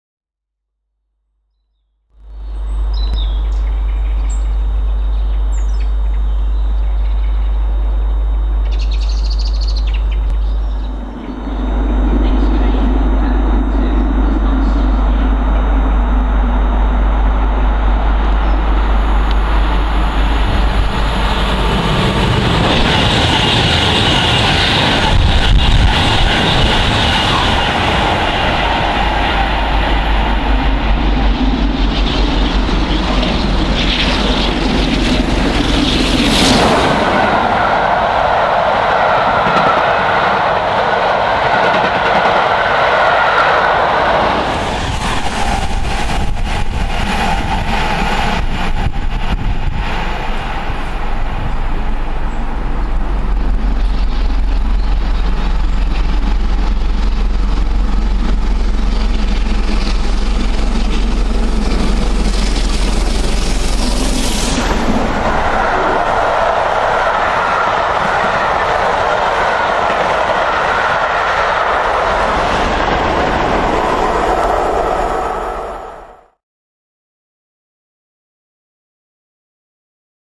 LMS 46233 Rips Through Hartford Station - The Appleby Explorer
46233, Duchess Of Sutherland, makes a fine passage through Hartford Station, on the Appleby Explorer. Unfortunately the approach view is slightly restricted due to a passing Pendolino. Hope you enjoy.